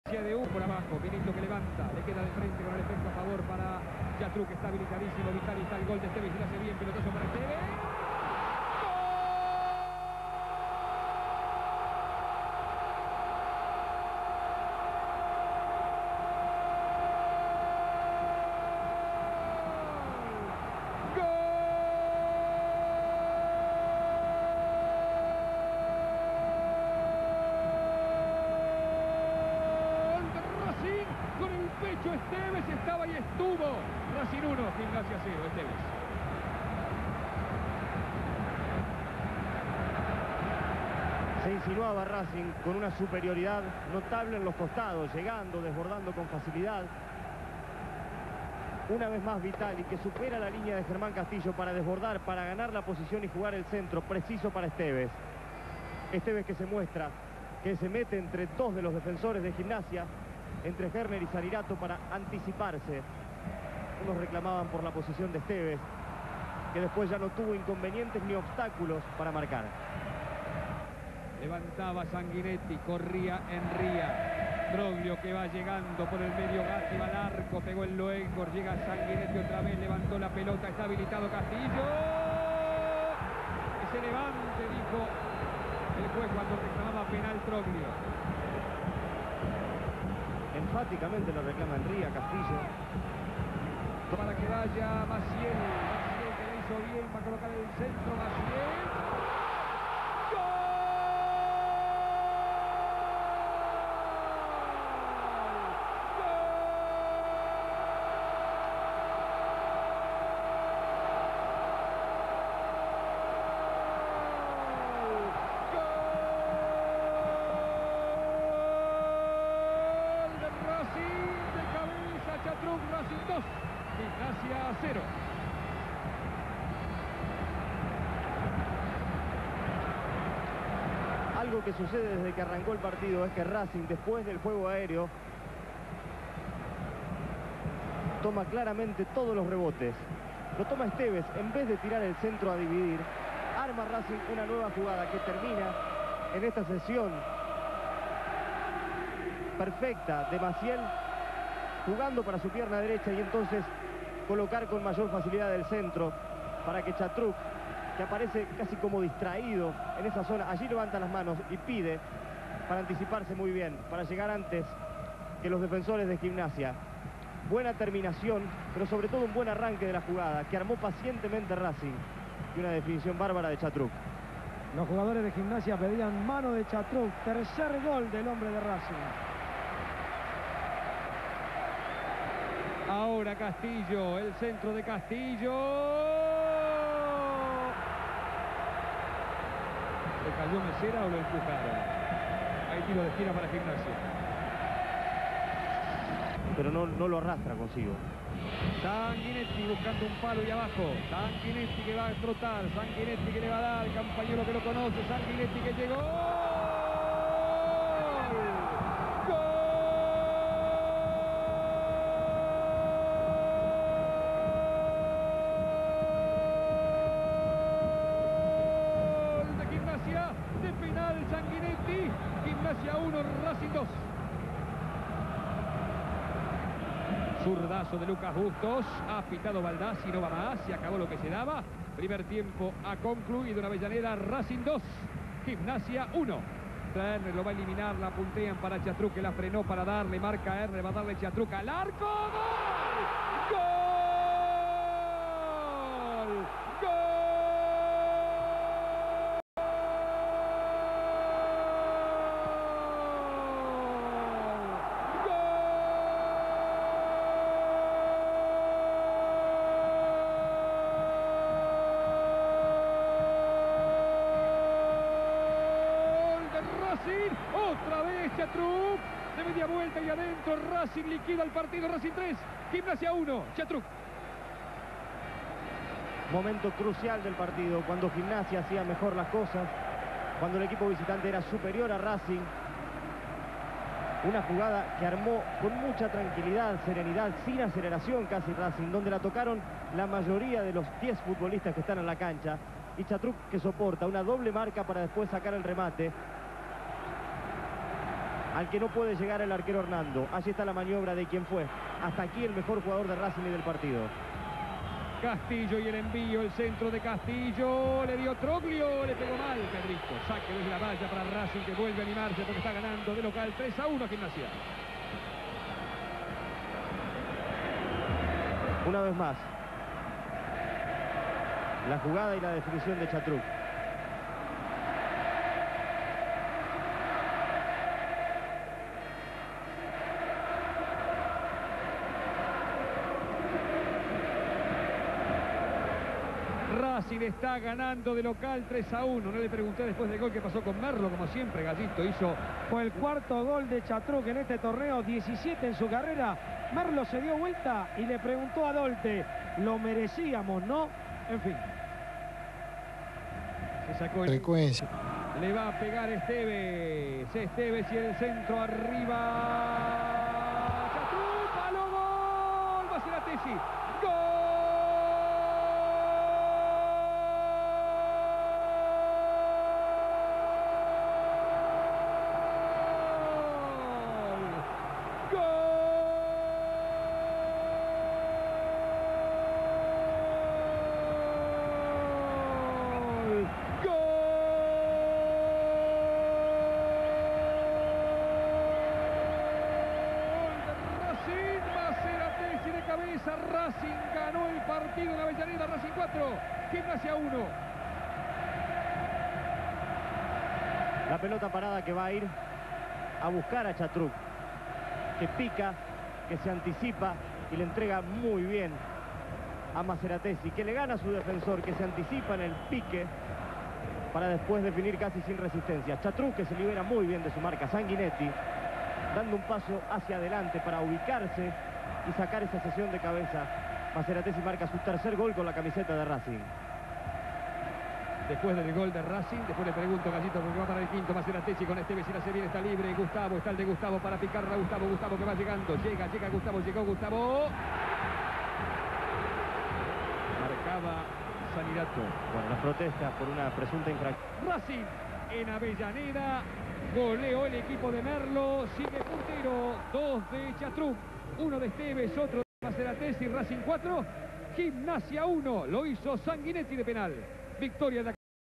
4-11-2001 (Apertura) (12°F) Racing Club:4 vs Gimnasia (LP):1
Goles:Estevez-Chatruc-Sanguinetti-Chatruc-Maceratesi.